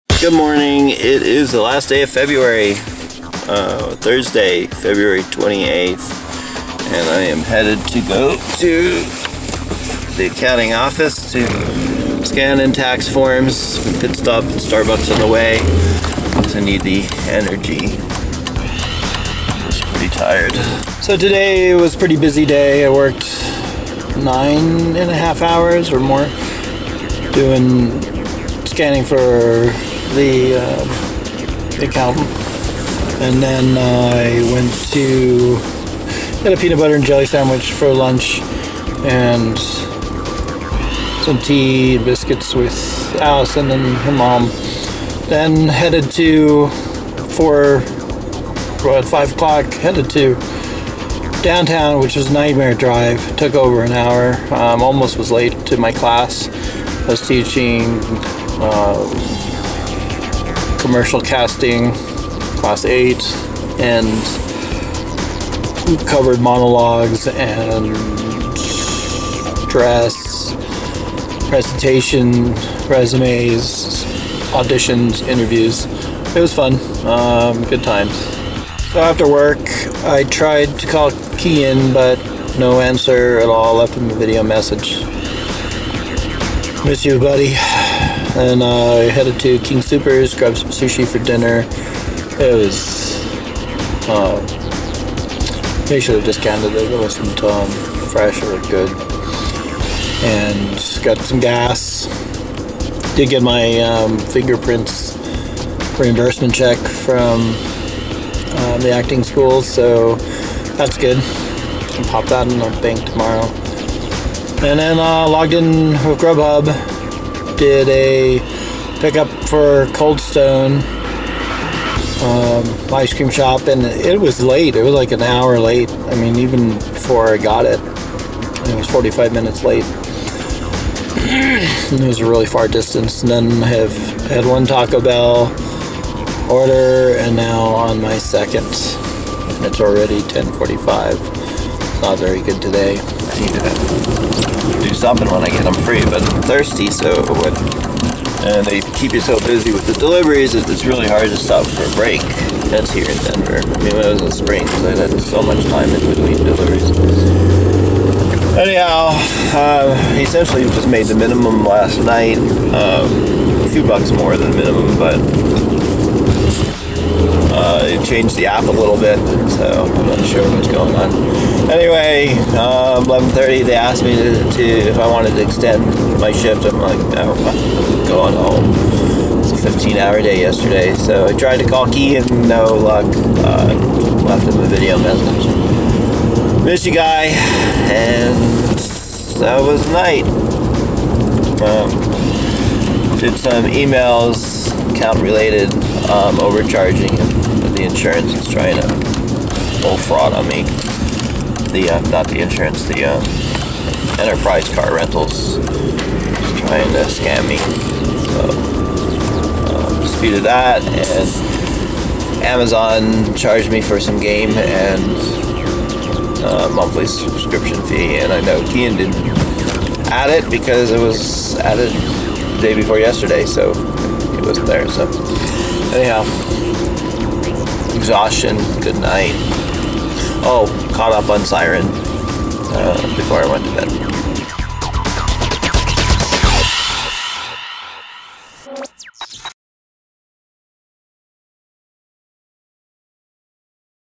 Thursday February 28, 2019: back to the grind
Chronicles of Sir Oisin Leif on Thursday, February 28, 2019 doing deliveries, teaching Commerical casting at Acting school, scanning tax documents for a local accounting office. Ponderings. Video Blog